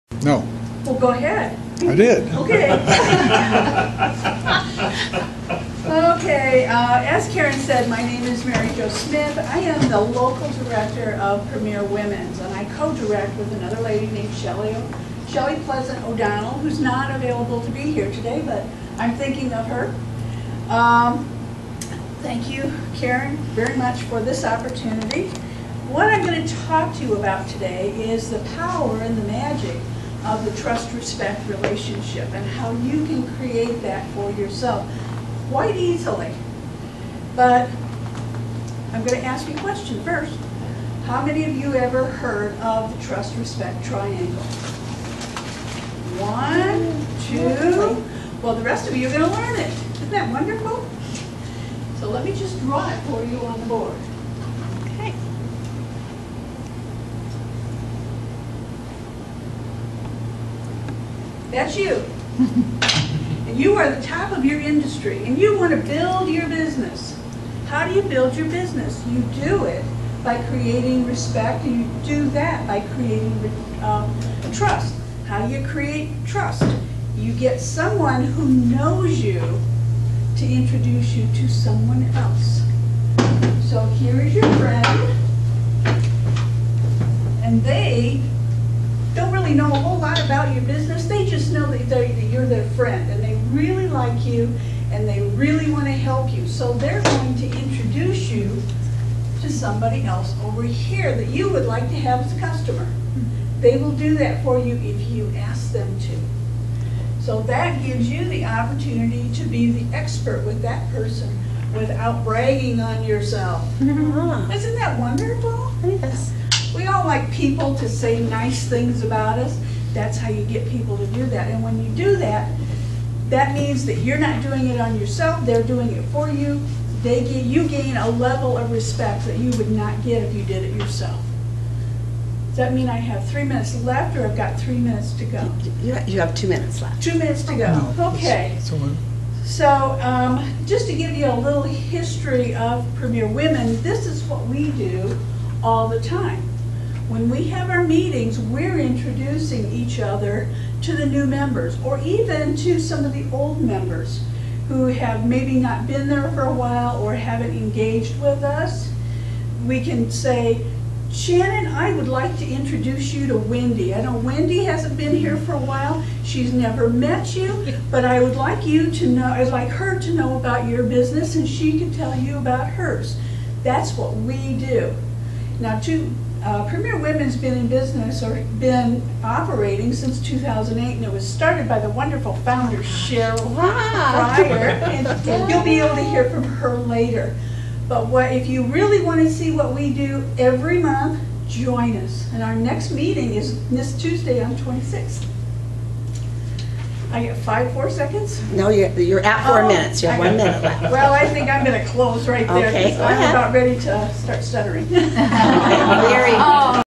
Mary Jo Smith
Women in Networking - How to Use the Trust, Respect Triangle to Grow Your Business